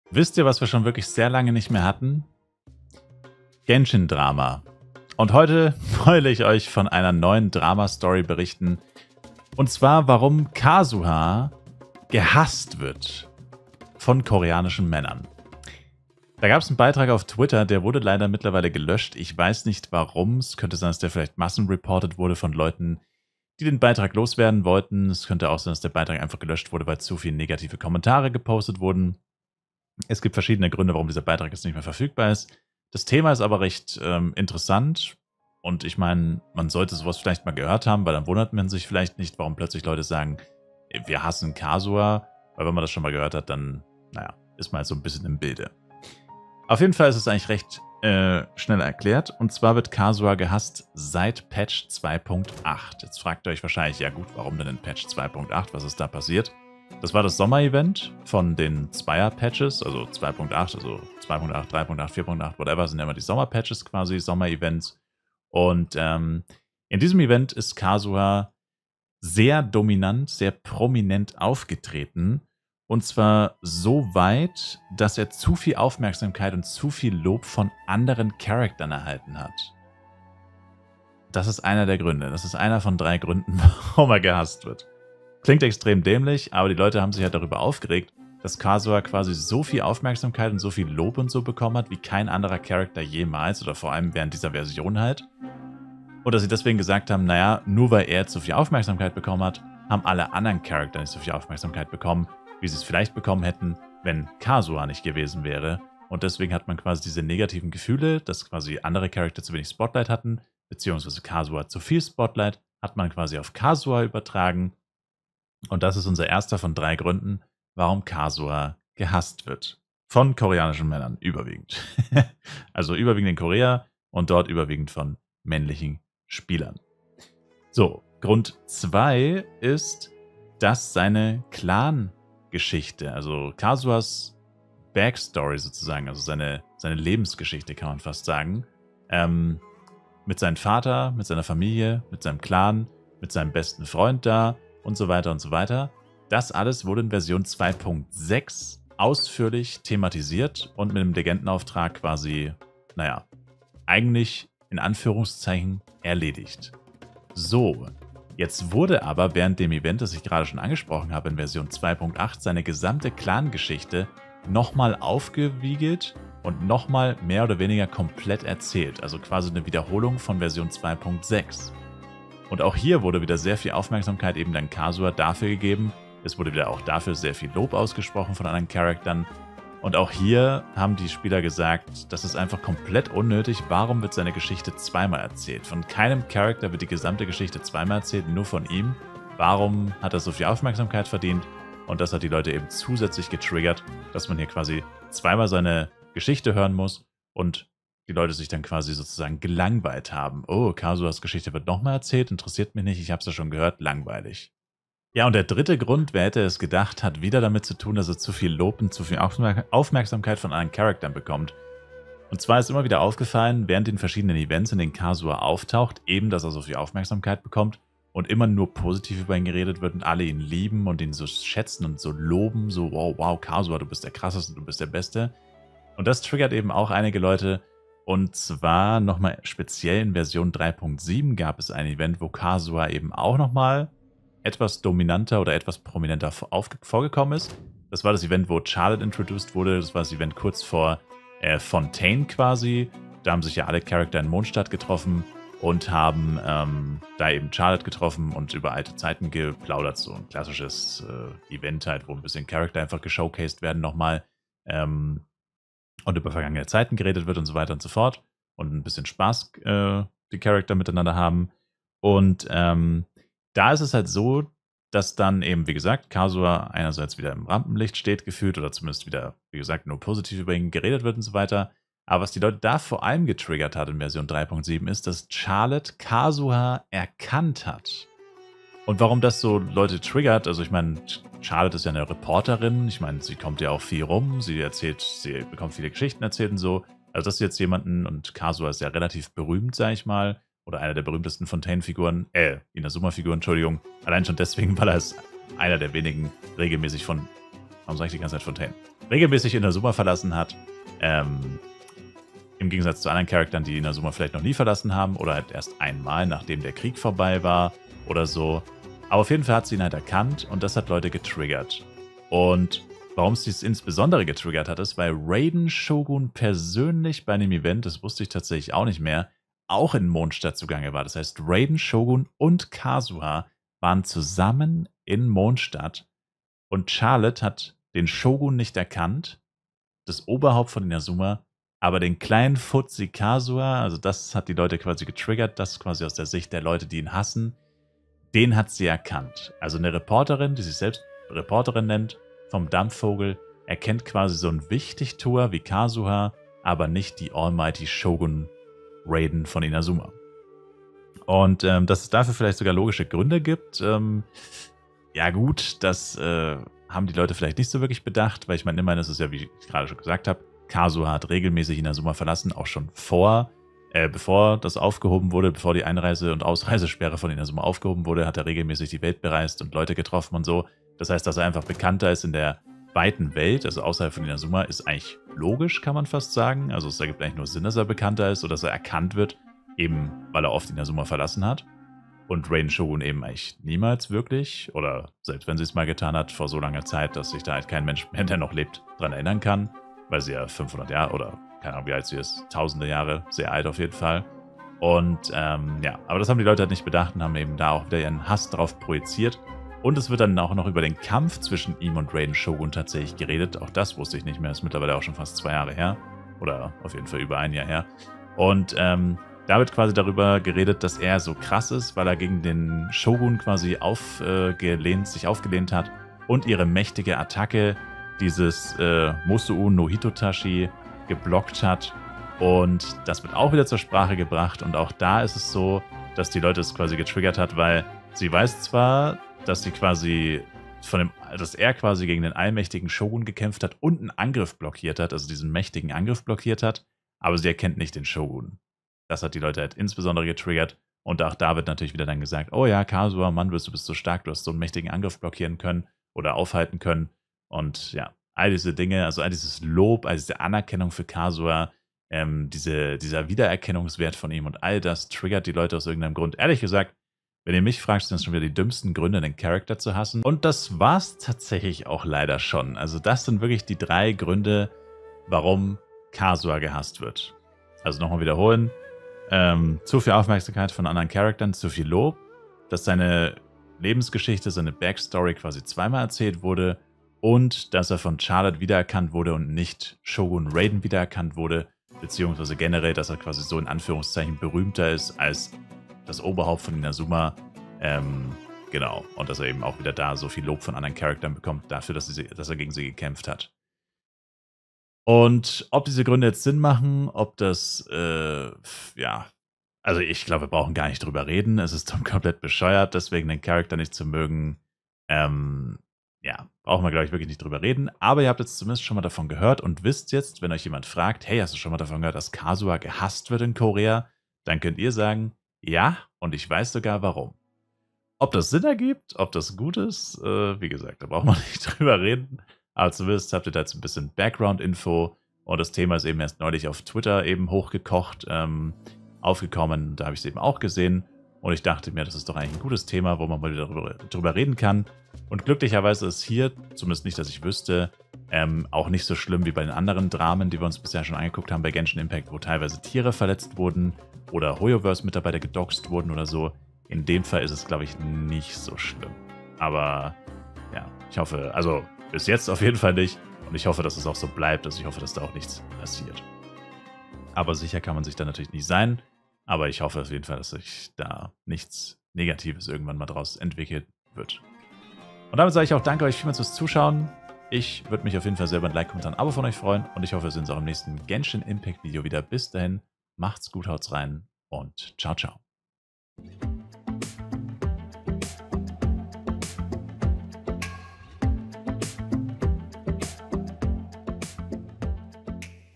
DESHALB wird Kazuha in Korea GEHASST! Genshin Impact Drama
Hauptkanal:  @Spielestyler
Zweitkanal:  @spielestylertv